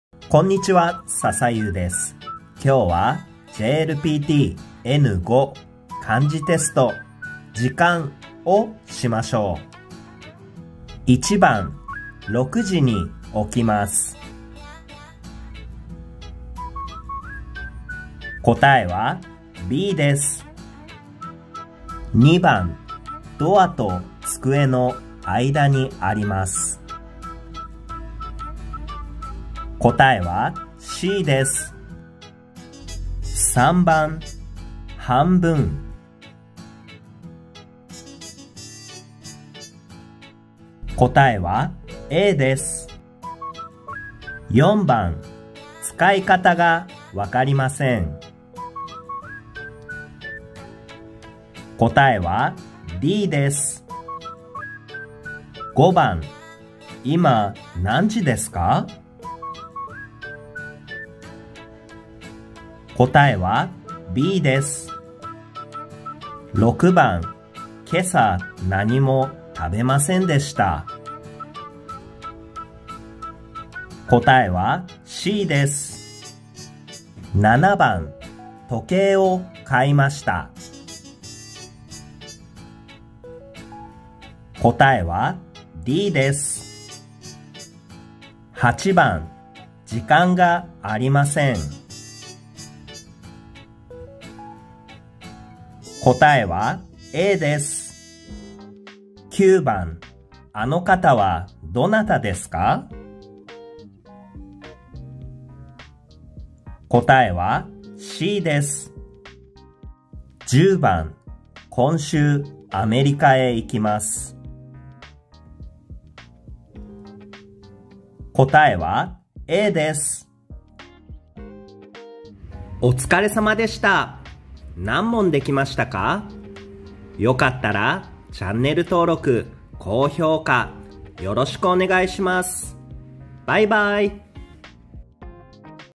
JLPT N5 【Kanji Test - Time】#7 漢字テスト「時間」
かんじテストを　しましょう。
きょうは 「じかん」のテストです。